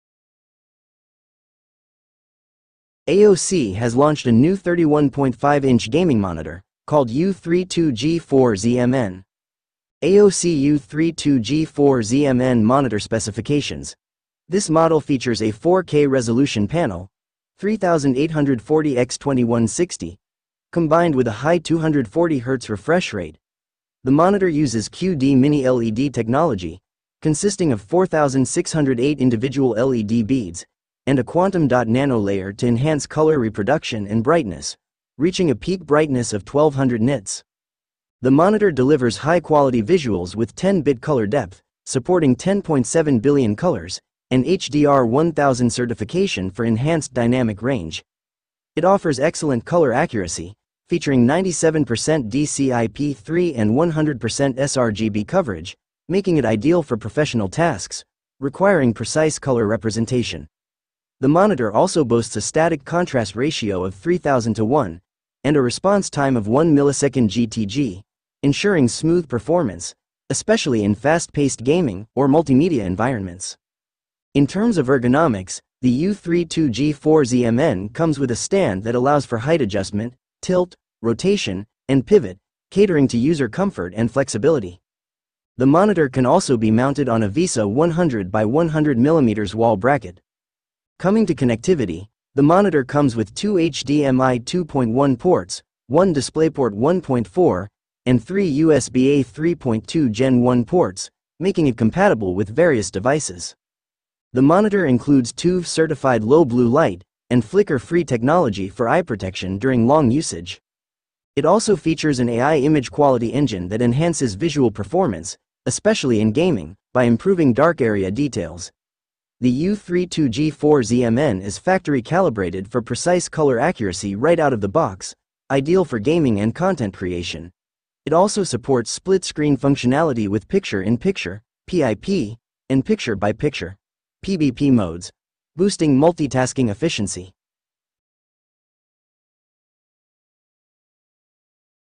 AOC U32G4ZMN Gaming Monitor : First Look - Review Full Specifications
AOC has launched a new 31.5-inch gaming monitor called U32G4ZMN in the Chinese market. The monitor is priced at 5,499 yuan ($776) and is now available on JD.com. Let’s check out its key specs.